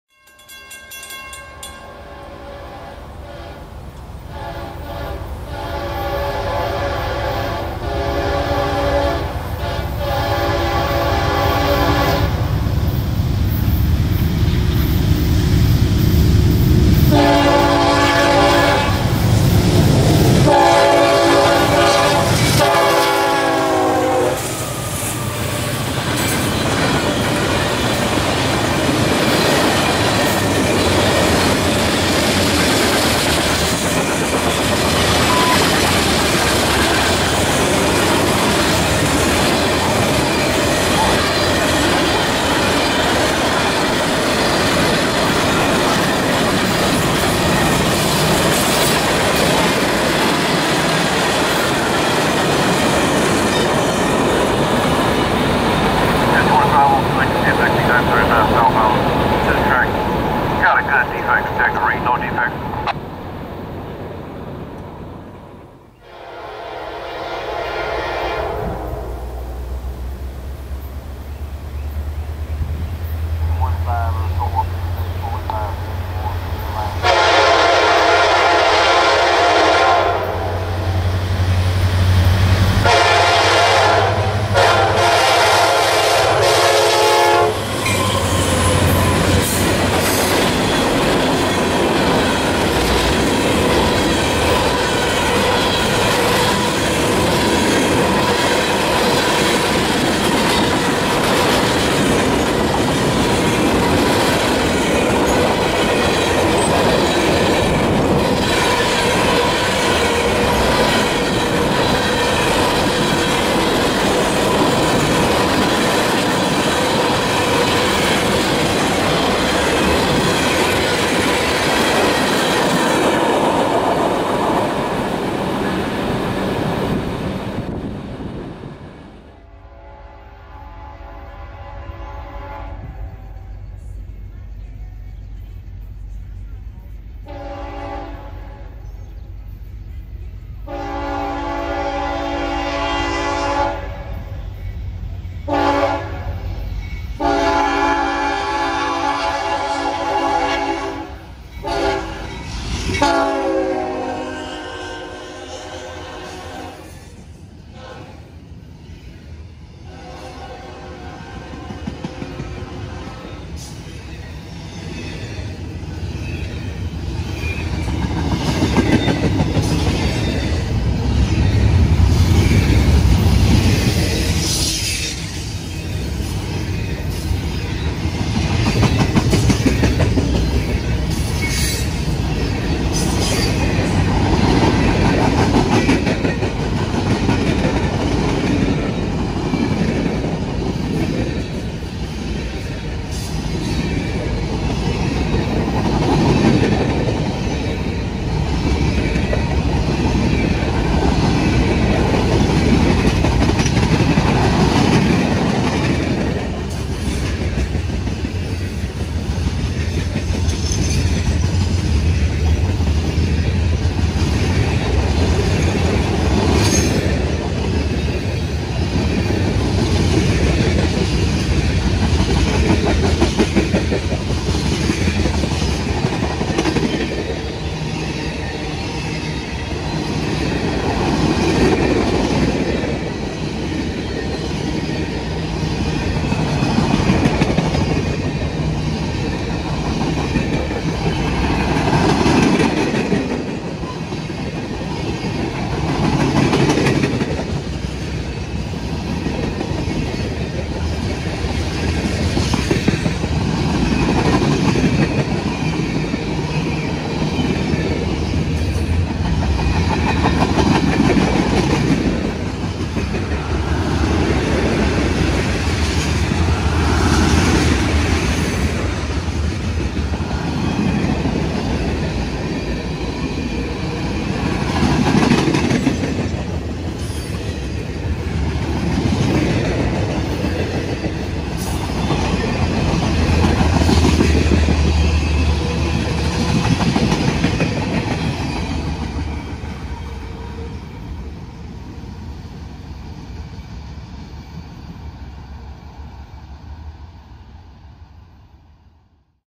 C40-9W/SD70MAC Pair on the Wilmington Locals
Here we see L225 and L607 with an SD70MAC and YN2 C40-9W pair.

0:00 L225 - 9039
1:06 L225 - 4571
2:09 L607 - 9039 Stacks